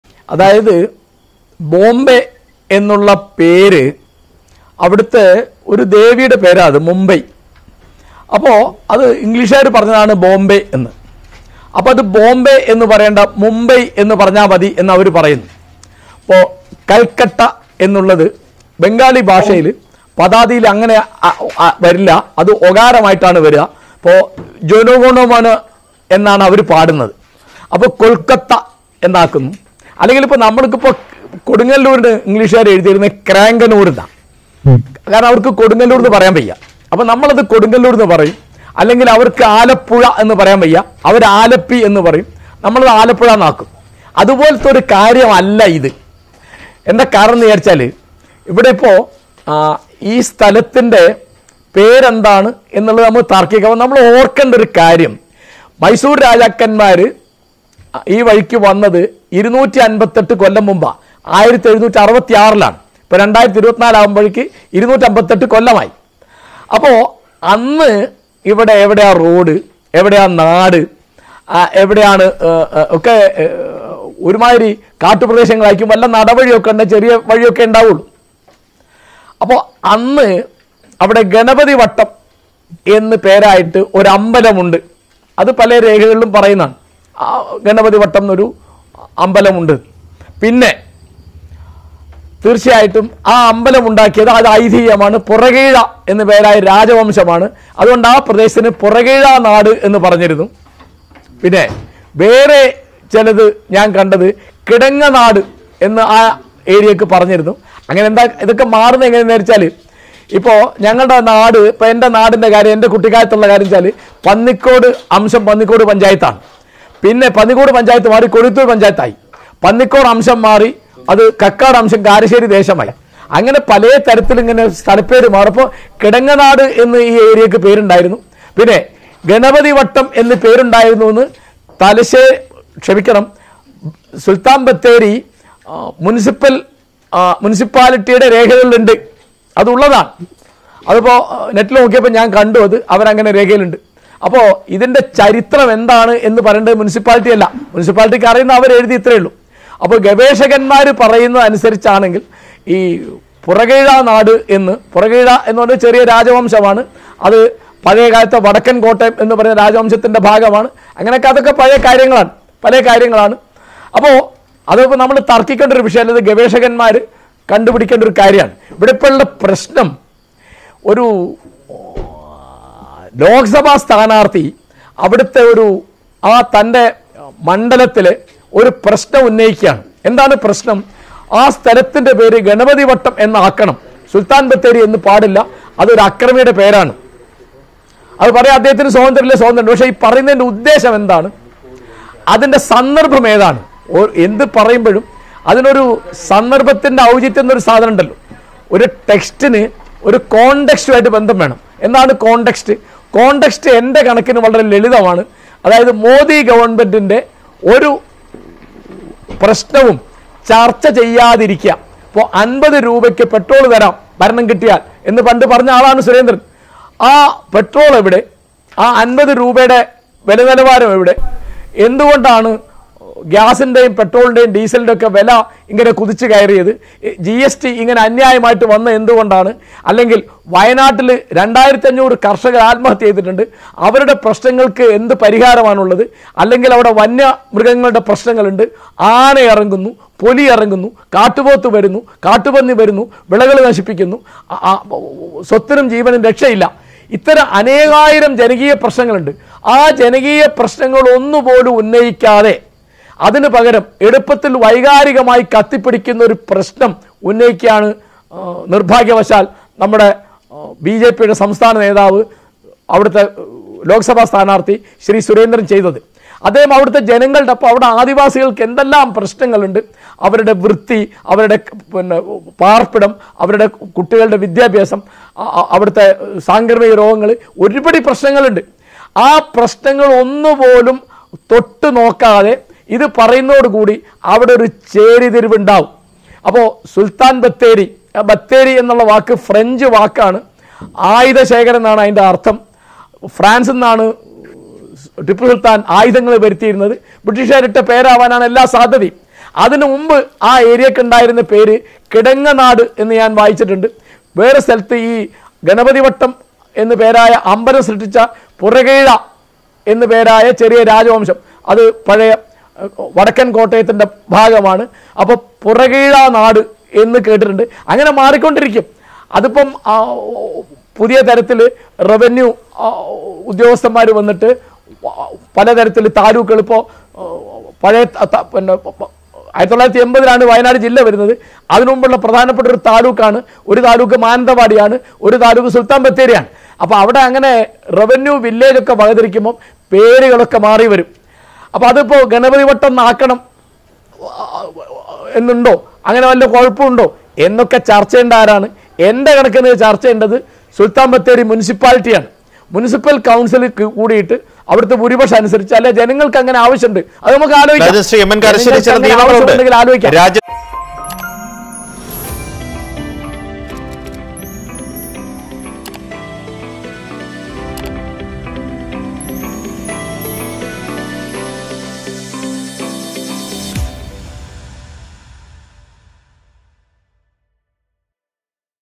'എളുപ്പത്തിൽ വൈകാരികമായി കത്തിപ്പിടിക്കുന്ന പ്രശ്നമാണ് ബിജെപി സ്ഥാനാർത്ഥി ഉന്നയിച്ചത്'
'വയനാട്ടിൽ വന്യമൃ​ഗ ശല്യം, കർഷക ആത്മഹത്യയടക്കം അനേകായിരം ജനകീയ പ്രശന്ങ്ങളുണ്ട്', ഇവയൊന്നും ഉന്നയിക്കാതെ എളുപ്പത്തിൽ വൈകാരികമായി കത്തിപ്പിടിക്കുന്ന പ്രശ്നമാണ് ബിജെപി സ്ഥാനാർത്ഥി ഉന്നയിച്ചതെന്ന് എം എൻ കാരശ്ശേരി